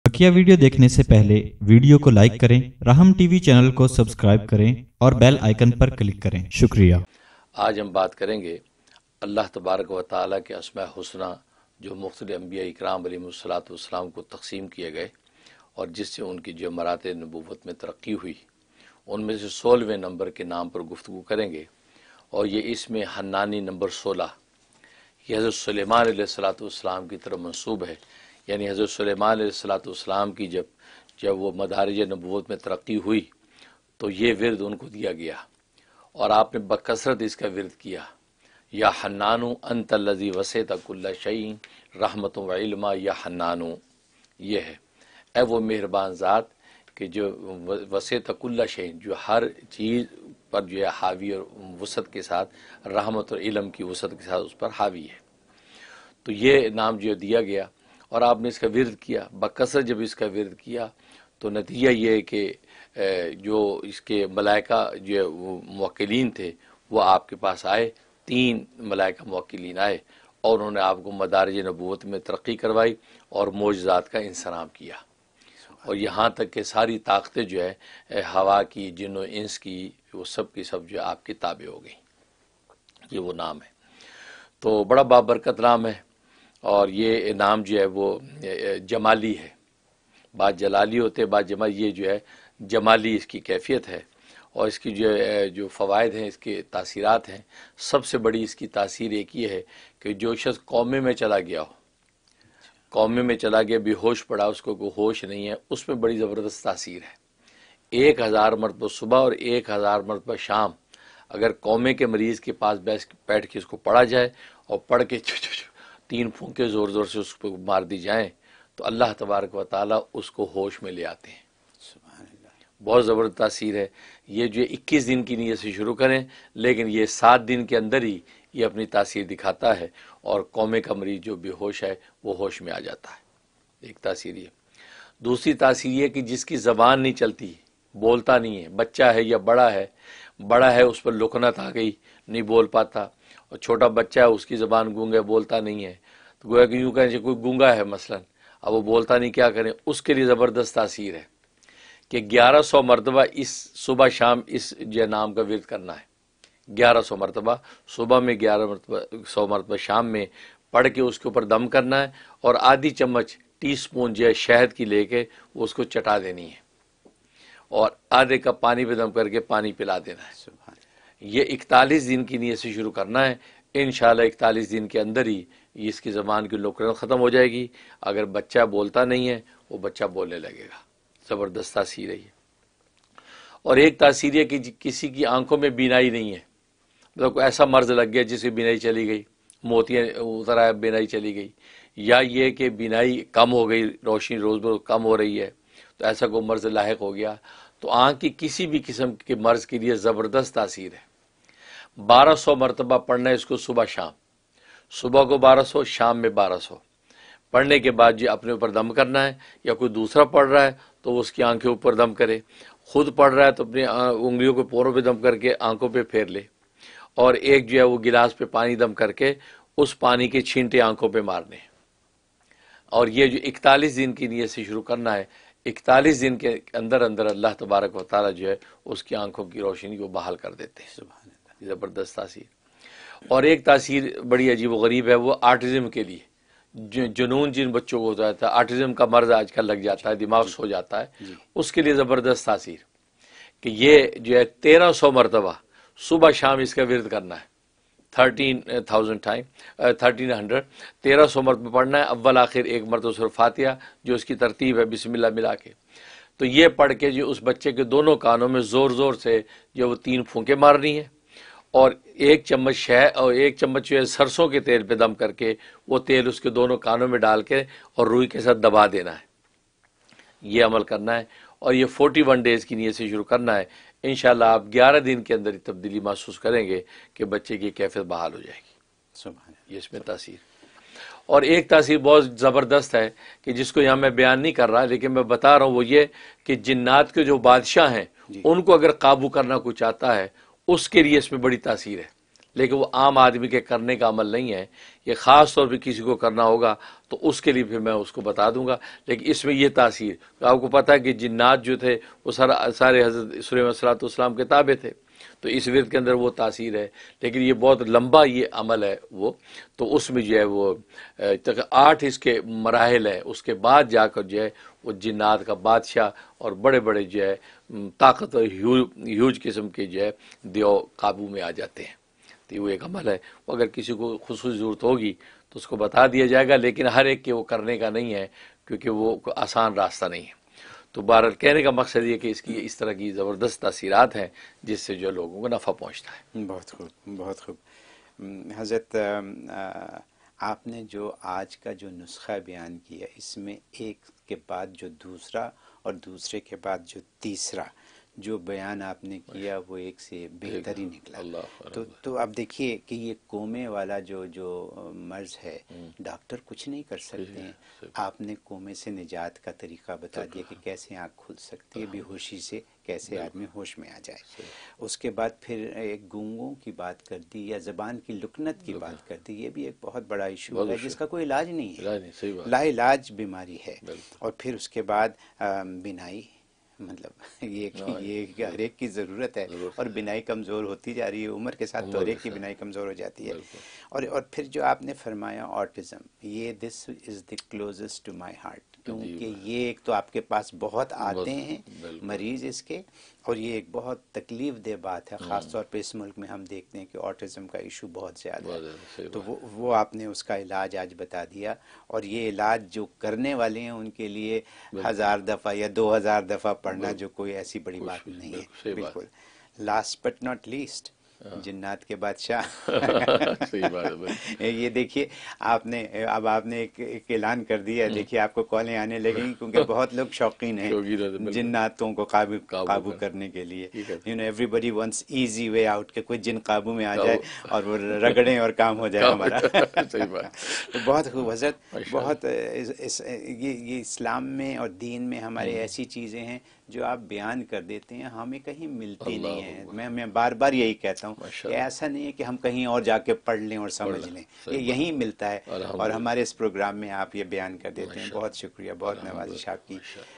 #16 Ism-e-Azam Hannani given to Hazrat Sulaimanؑ || Heal Coma, Mutism, Blindness || Control Jinnat!!
Ya Hannanu antal lazi wasi’at kula shayin rahmatan wa ilman ya Hannanu

يَاحَنَّانُ اَنْتَ الَّذِى وَسِعْتَ كَلَّ شَيْئٍ رَّحْمَةً وَعِلْمًا يَاحَنَّانُ

Power to revive person from coma || Power to cure Mutism, Blindness, Insanity, Autism etc. || Power to control Jinnat

Mufti Muneer Ahmed Akhoon -  Raham TV